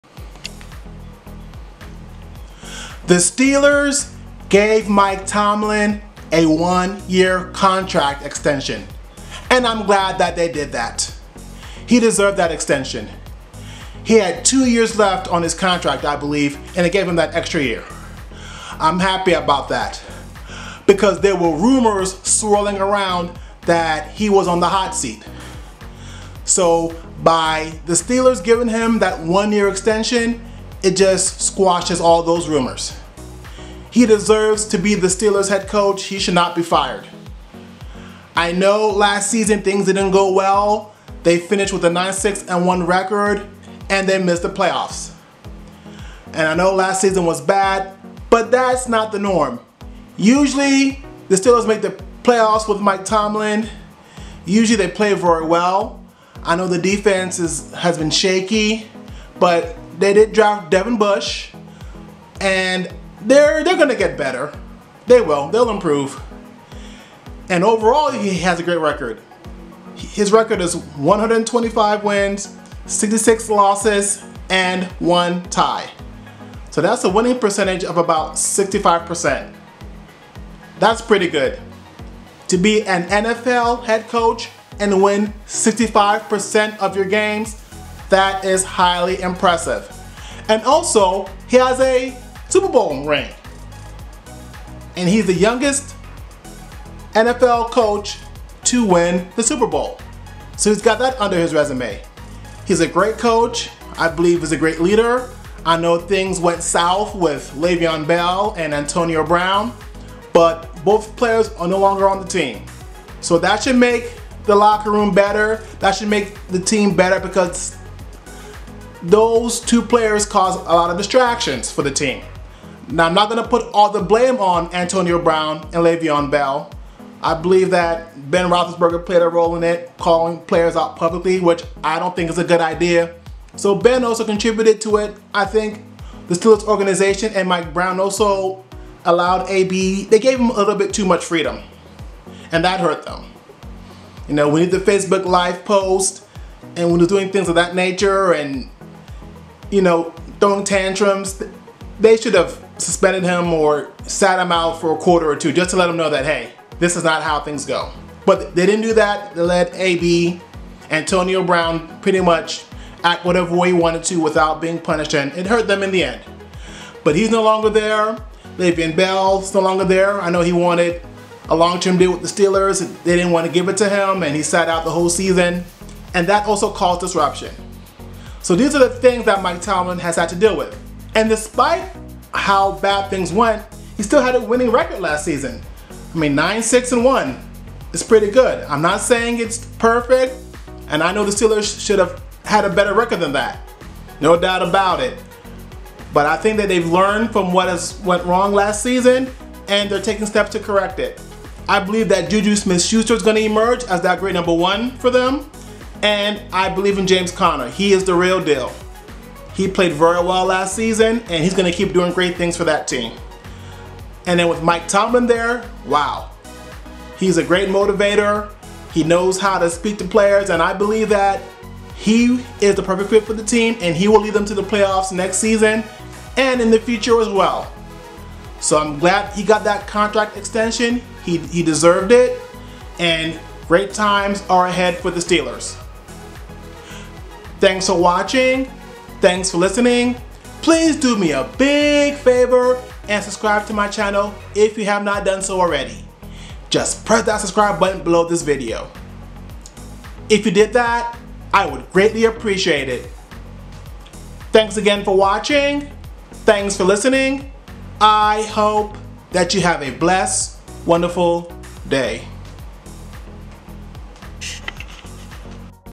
The Steelers Give Mike Tomlin a Contract Extension
In this video, I share why Mike Tomlin deserved a contract extension from the Pittsburgh Steelers

For Amazon Best Sellers in DSLR Cameras:

For Amazon Deep Discounts on Open -box and Used Camera Equipment:

Amazon Music Free 30-Day Trial: